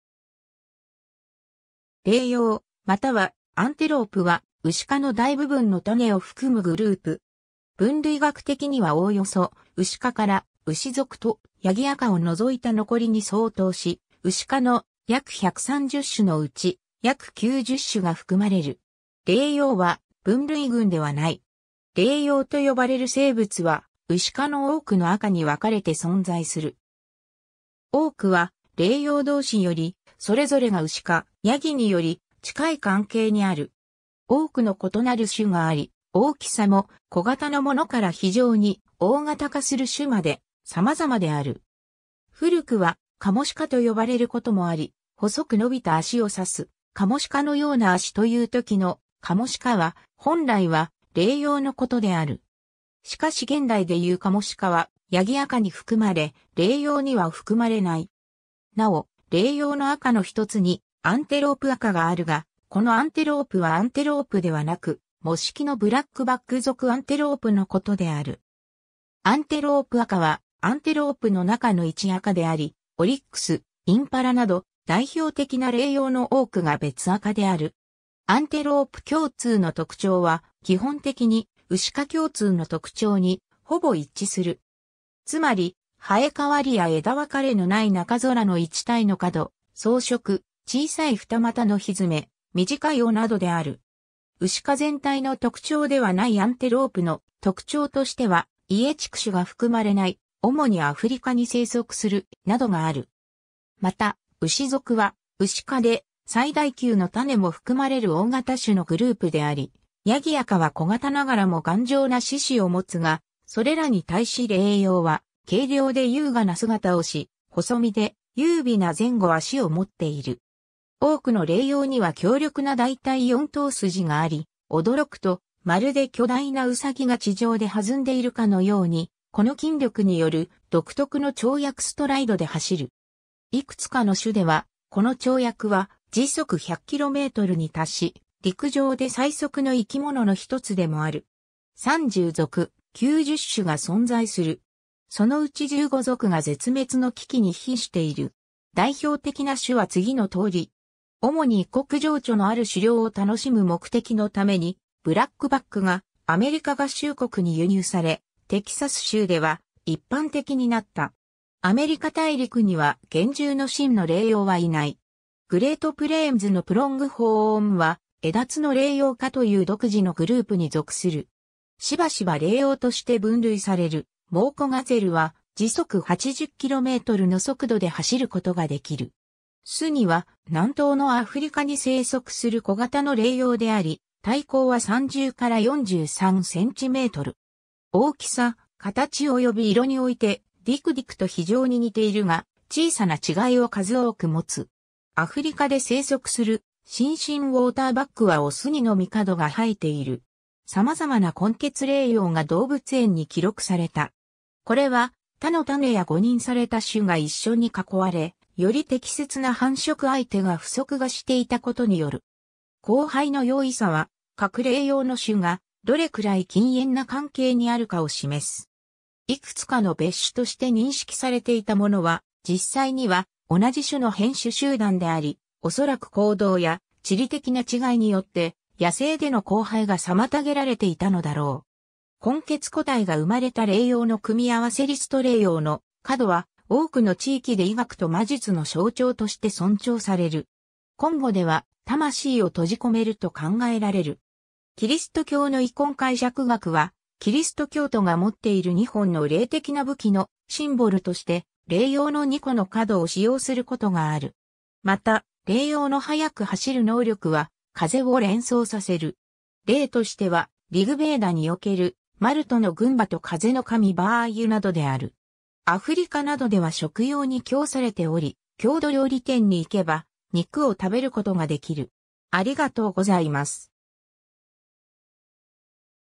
レイヨウ
レイヨウ（羚羊）またはアンテロープ (Antelope) は、ウシ科の大部分の種を含むグループ。
分類学的にはおおよそ、ウシ科からウシ族とヤギ亜科を除いた残りに相当し、ウシ科の約130種のうち約90種が含まれる。
 「レイヨウ」は分類群ではない。
レイヨウと呼ばれる生物は、ウシ科の多くの亜科（ヤギ亜科以外の全て）に分かれて存在する。
多くはレイヨウ同士より、それぞれがウシかヤギにより近い関係にある。
多くの異なる種があり、大きさも、小型のものから非常に大型化する種まで、さまざまである。
 古くは「カモシカ」と呼ばれることもあり、細く伸びた足を指す「カモシカのような足」というときの「カモシカ」は、本来はレイヨウのことである。
しかし現代でいうカモシカはヤギ亜科に含まれ、レイヨウには含まれない。
 なお、レイヨウの亜科のひとつにアンテロープ亜科（ブラックバック亜科）があるが、このアンテロープはAntelopeではなく、模式のブラックバック属 Antilope のことである。
アンテロープ亜科はアンテロープの中の1亜科であり、オリックス、インパラなど代表的なレイヨウの多くが別亜科である。
 アンテロープ共通の特徴は、基本的に、ウシ科共通の特徴にほぼ一致する。
つまり、生え変わりや枝分かれのない中空の1対の角、草食、小さい二股の蹄、短い尾などである。
 ウシ科全体の特徴ではないアンテロープの特徴としては、家畜種が含まれない、主にアフリカに生息する、などがある。
 また、ウシ族はウシ科で最大級の種も含まれる大型種のグループであり、ヤギ亜科は小型ながらも頑丈な四肢を持つが、それらに対しレイヨウは、軽量で優雅な姿をし、細身で、優美な前後脚を持っている。
 多くのレイヨウには強力な大腿四頭筋があり、驚くとまるで巨大なウサギが地上で弾んでいるかのように、この筋力による独特の跳躍ストライドで走る。
 いくつかの種では、この跳躍は100 km/hに達し（チーターの100-115 km/hに匹敵し、しかも持久力では勝る）、陸上で最速の生き物の1つでもある。
 30属・90種が存在する。
そのうち15属が絶滅の危機に瀕している。
 代表的な種は次のとおり。
 主に異国情緒のある狩猟を楽しむ目的のために、ブラックバックがアメリカ合衆国に輸入され、テキサス州では一般的になった。
アメリカ大陸には原住の真のレイヨウはいない。
グレートプレーンズのプロングホーンは、エダツノレイヨウ科(Antilocapridae)という独自のグループに属する。
しばしばレイヨウとして分類されるモウコガゼル(Procapra gutturosa)は、80 km/hの速度で走ることができる。
スニは南東のアフリカに生息する小型のレイヨウであり、体高は30-43 cm。
大きさ・形および色において、ディクディク(Dik-dik)と非常に似ているが、小さな違いを数多くもつ。
アフリカで生息するシンシンウォーターバックはオスにのみ角が生えている。
 さまざまな混血レイヨウが動物園に記録された。
これは、他の種や誤認された種が一緒に囲われ、より適切な繁殖相手が不足がしていたことによる。
交配の容易さは、各レイヨウの種がどれくらい近縁な関係にあるかを示す。
いくつかの別種として認識されていたものは、実際には同じ種の変種集団であり、おそらく行動や地理的な違いによって野生での交配が妨げられていたのだろう。
 混血個体が生まれたレイヨウの組合せリスト レイヨウの角は、多くの地域で医学と魔術の象徴として尊重される。
 コンゴでは、魂を閉じ込めると考えられる。
 キリスト教のイコン解釈学は、キリスト教徒が持っている2本の霊的な武器(旧約聖書と新約聖書)のシンボルとして、レイヨウの2個の角を使用することがある。
また、レイヨウの速く走る能力は、風を連想させる。
例としては、『リグ・ヴェーダ』におけるマルトの軍馬と風の神ヴァーユなどである。
 アフリカなどでは食用に供されており、郷土料理店に行けば肉を食べることが出来る。